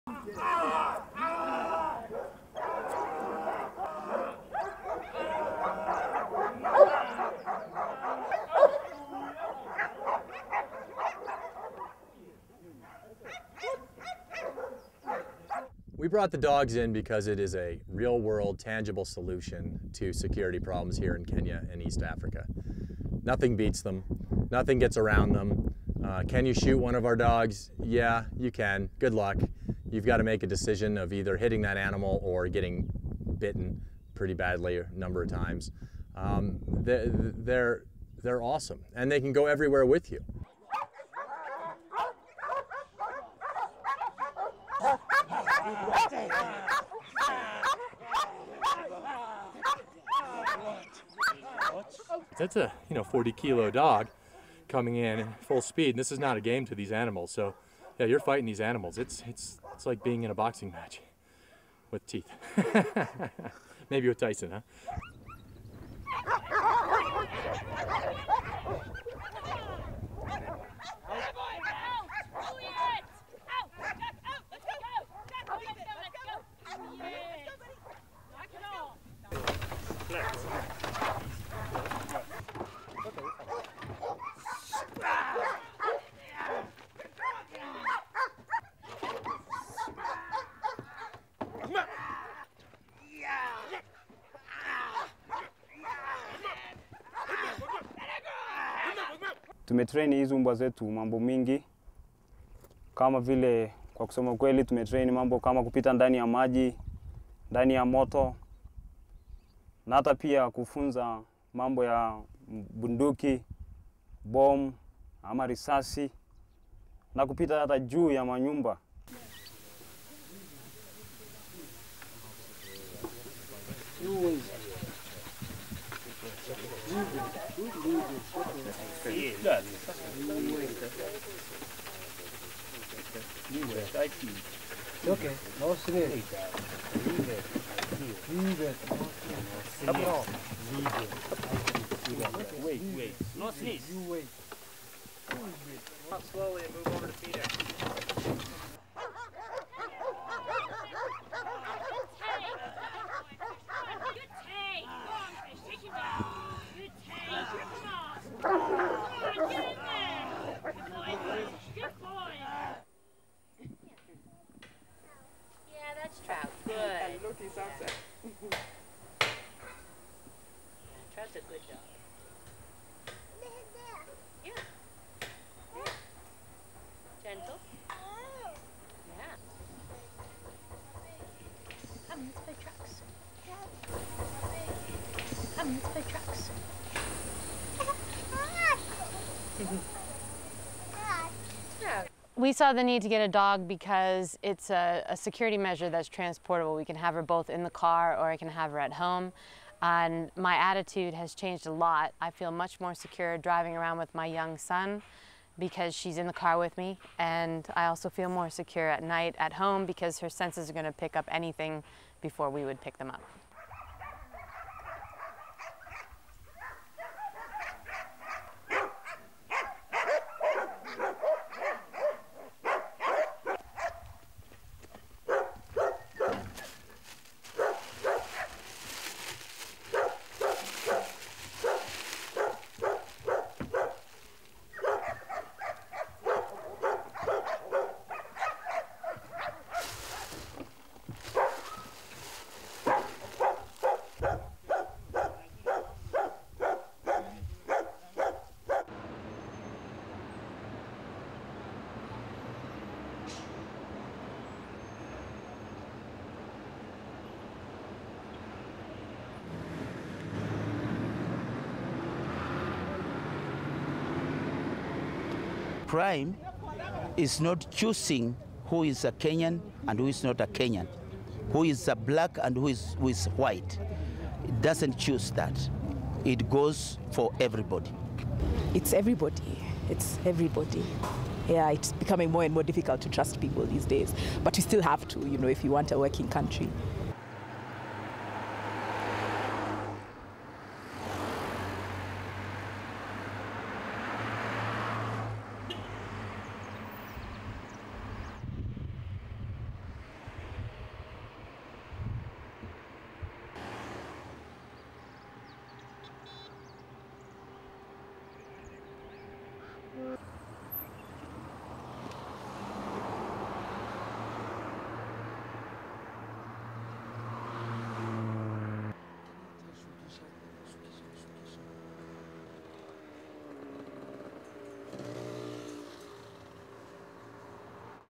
Guard dogs trained to crack rising crime
Naivasha, Kenya - 8 January, 2010
1. Wide shot of dogs attacking.
2. Mid shot of dogs attacking.
3. Tilt up of dogs releasing attacker.
4. SOUNDBITE: (English) Jeff Greene, CEO Ridgeback
"We brought the dogs in because it is a real world tangible solution to security problems here in Kenya and East Africa. Nothing beats them, nothing gets around them. Can you shoot one of our dogs? Yeah, you can, good luck. You have got to make a decision of either hitting that animal or getting bitten pretty badly a number of times. They are awesome and they can go everywhere with you."
5. Wide shot of Jeff Greene performing a dog attack drill.
6. Mid shot of dogs attacking.
7. UPSOUND: (English) Jeff Greene, CEO Ridgeback
"That''s a forty kilo dog coming at you with full speed. This is not a game to these animals, so you are fighting these animals. It''s like being in a boxing match with teeth... Maybe with Tyson, huh?."
Naivasha, Kenya - 7 December 2009
8. Wide shot of dogs in bite work drill.
9. Mid shot of dogs in bite work drill.
10. Wide shot of dogs in obstacle drills.
11. Mid shot of dog jumping over set up obstacle.
12. Mid shot of dog and trainer jumping over a set up obstacle.
13. Mid shot of dogs attacking.
14. SOUNDBITE: (Swahili) Stanley Kuyon, dog trainer
"We have trained these dogs extensively. We have trained the dogs to swim under water, deploy into fire as well as teach them about guns, bombs and bullet firing scenarios, and even to cross over rooftops."
15. Various shots of height endurance dog training.
16. Wide shot of dog deploying from a vehicle.
17. Mid shot of dog attacking.
Nairobi, Kenya - 9 December, 2009
18. Various shots of Stephanie Dloniak and her son Oliver with their dog, Trout.
19. SOUNDBITE: (English) Stephanie Dloniak, an owner of Ridgeback protection dog,
"We saw the need to get a dog because it''s a security measure that is transportable. We can have her both in the car or I can have her at home and my attitude has changed a lot. I feel much more secure driving around with my young son because she is in the car with me and I also feel more secure at night at home because he senses are going to pick up anything before we would pick them up."
20. Various shots of dogs by their kennels eating.
Nairobi, Kenya - 22 December, 2009
21. High shot of Nairobi city centre.
22. Wide shot of Nairobi city centre.
23. Mid shot of city centre.
24. SOUNDBITE: (English) Edward Murila, Nairobi resident,
"Crime is not choosing who is a Kenyan and who is not a Kenyan. Who is black and who is white. It doesn''t choose that. It goes for everybody."
25. SOUNDBITE: (English) Njoki Ngumi, Nairobi resident,
"It''s everybody, it''s everybody. It is becoming more and more difficult to trust people these days, but we still have to if we want a working country."
Nairobi, Kenya - 19 January, 2010
26. Pan of alarm response security guards.
27. Mid shot of security guards.
28. Tilt up of security guard.
29. Wide shot of security guard opening security barrier.
30. Mid shot of security guard closing security barrier.
LEAD IN:
Nairobi can be a dangerous place to live.
Police figures for 2009 show that the numbers of kidnaps rose from about 3% - 19% of total crime that year.
While residents take precautionary security measures such as electric fences, guards and alarm systems, a Nairobi based security firm is turning man''s best friend to produce one of the best weapons yet, dogs.
STORYLINE:
These dogs are being trained to attack intruders.
The dogs are trained to react in a variety of stressful scenarios.